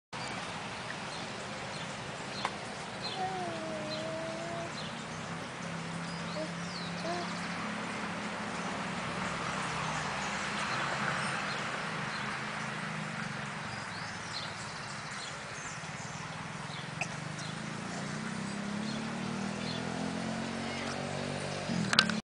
Drosera Capensis Alba + Mosquito - Plantas Carnivoras
la especie drosera Capensis Alba ha cazado un mosquito (zancudo).